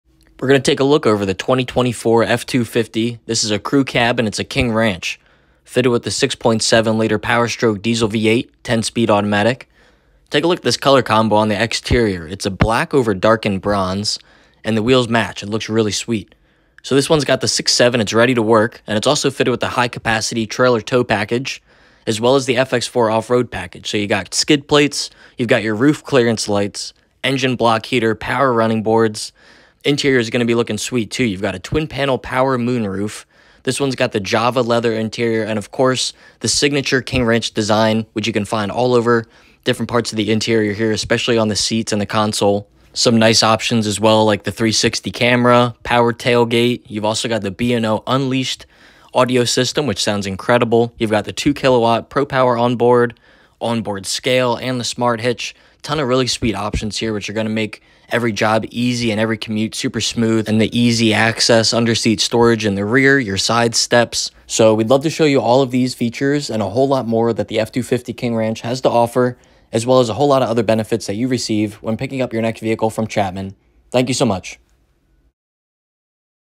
2024 Ford F250 Crew Cab King Ranch
2024 F-250 Crew Cab King Ranch with the 6.7L Powerstroke Diesel V8 10-Speed
Black over the darkened bronze exterior
High capacity trailer tow package
Fx4 off road package